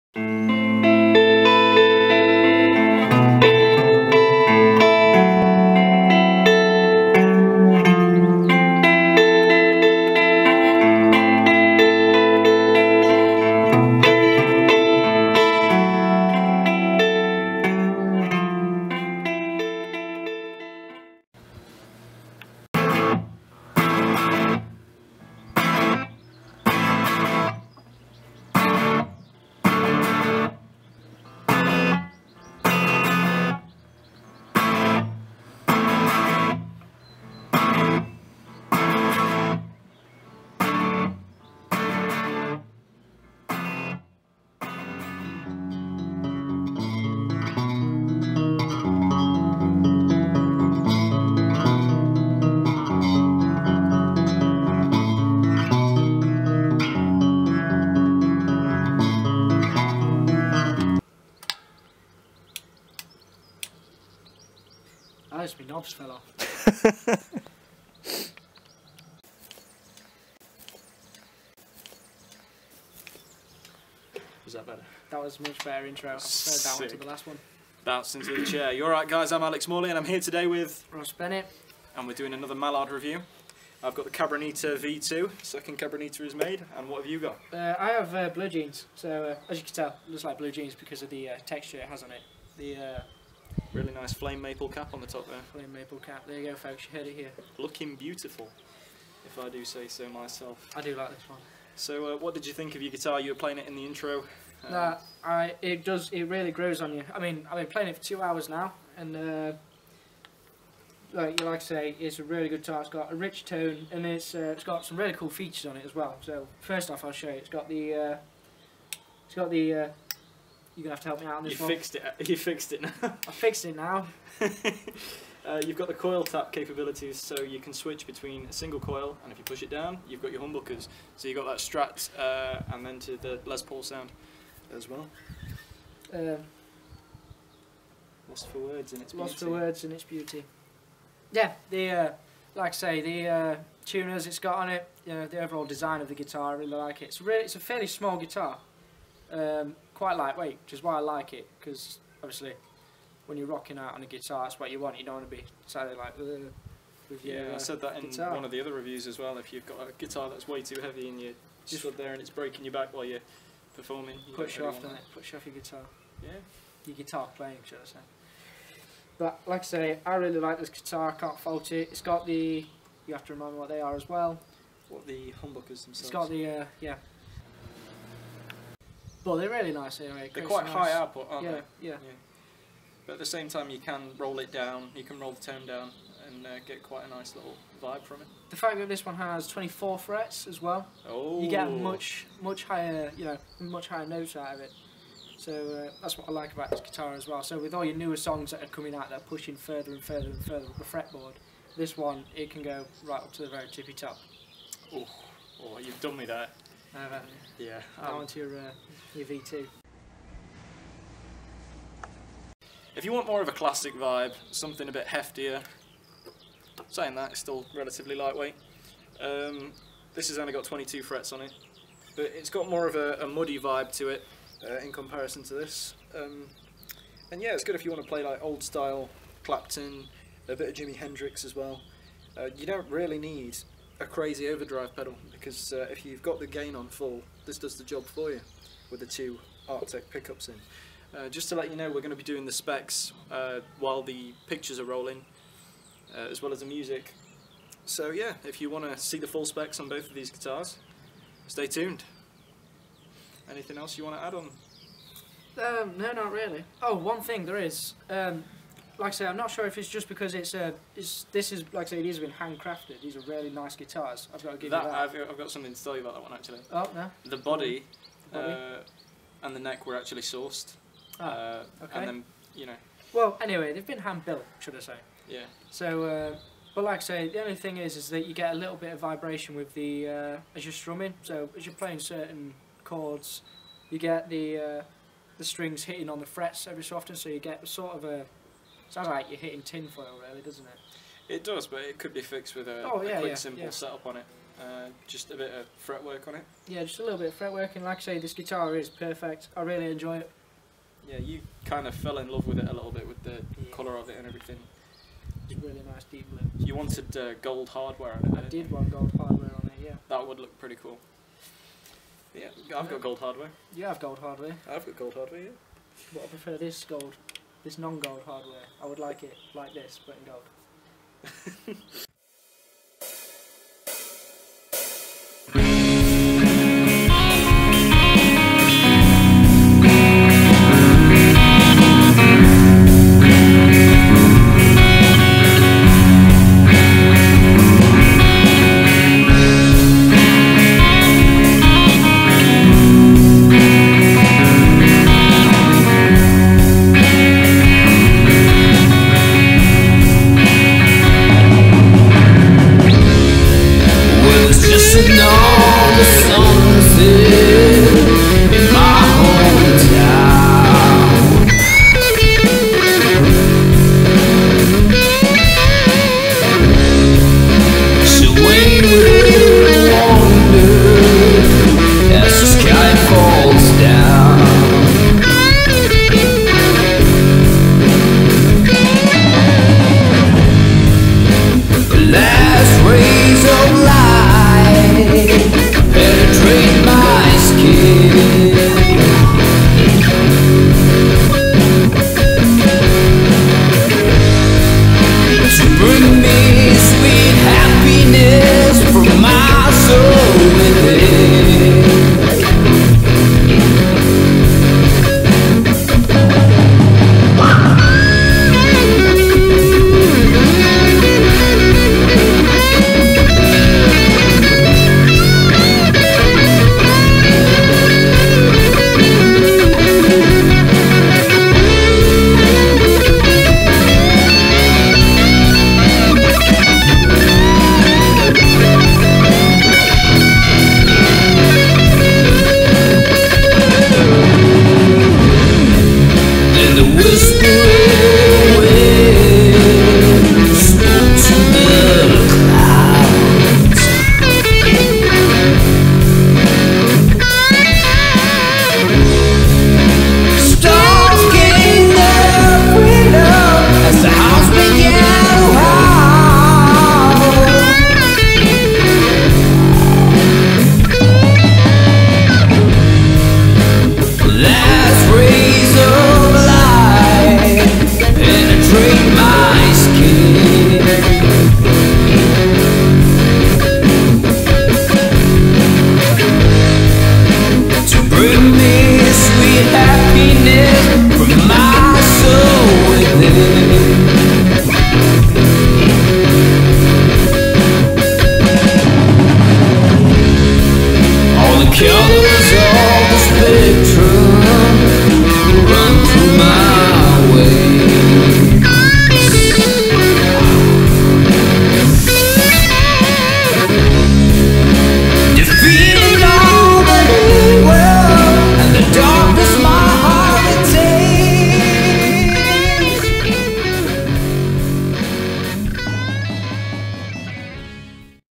Blue Jeans and Cabronita V2
Alex Morley and Ross Bennett review 2 new Mallard Guitars.